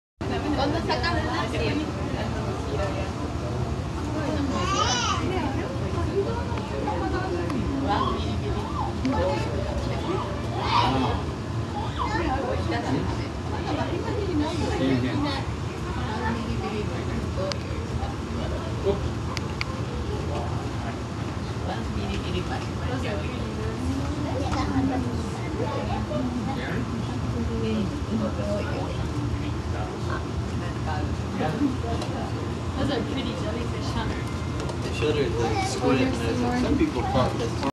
Creepy Jellyfish
I had no idea that these Jellyfish even existed.  They remind me of either tarantulas or mini-robots.  They move pretty fast, and it looks like they have these mini pearls/ball bearings on their hats.